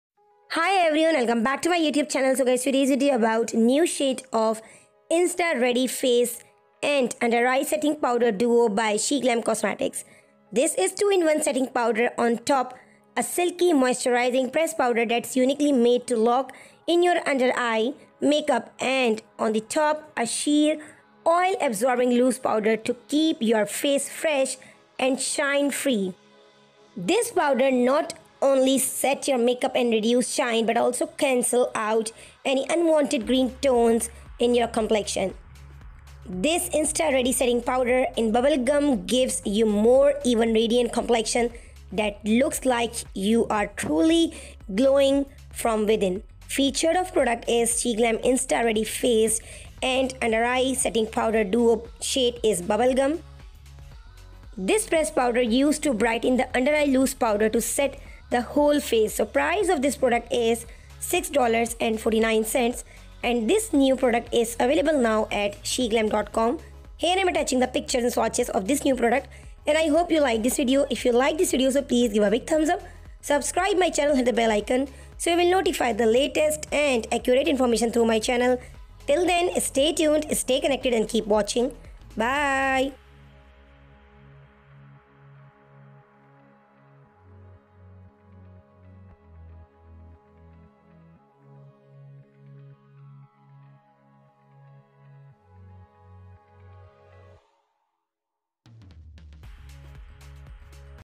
New!Shade of Insta-Ready Face & Under Eye Setting Powder Duo by SheGlam Cosmetics|New MakeupReleases
2-in-1 setting powder, on top a silky, moisturizing pressed powder that's uniquely made to lock in your under eye makeup and on the bottom, a sheer, oil-absorbing loose powder to keep your face fresh and shine-free from AM to PM $6.49

Now in shade: 💗Bubblegum pressed powder use to brighten the undereye, loose powder to set the whole face